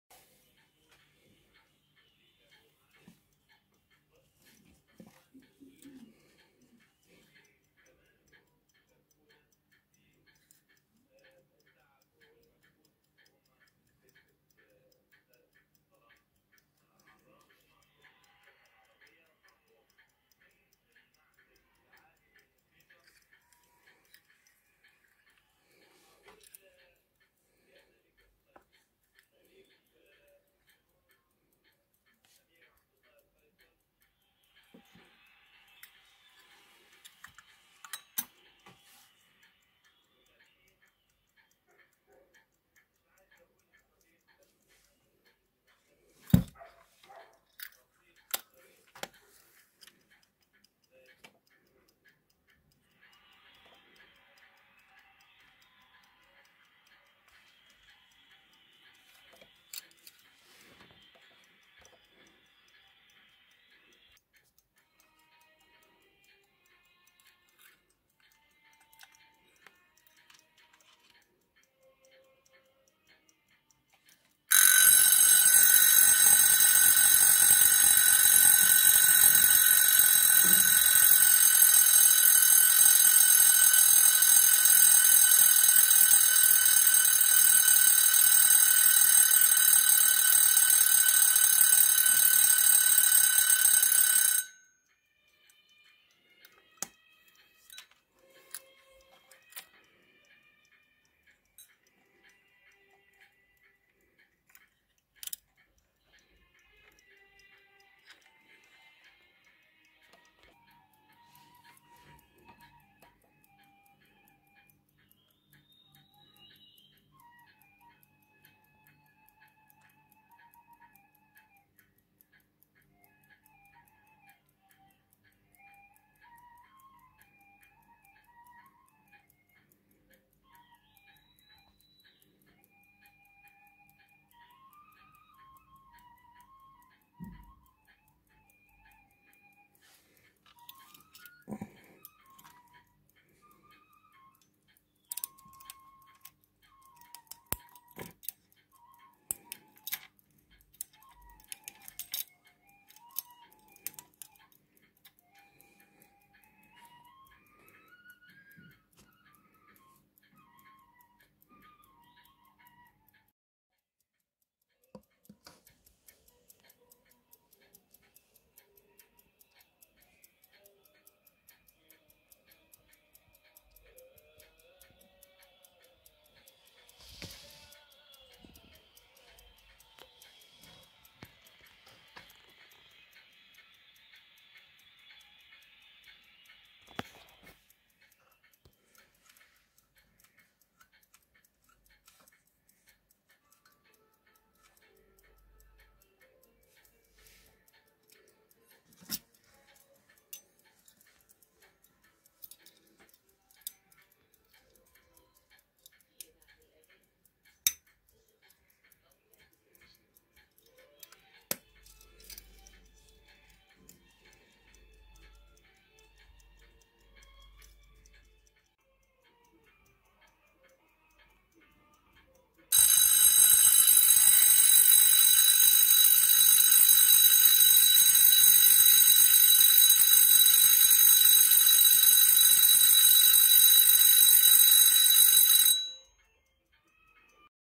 ساعة جيب نادرة جدا قلب مفتوح بجرس
ساعة جيب سويسرى بجرس وماكينة مكشوف من نوادر ساعات الجيب القديمة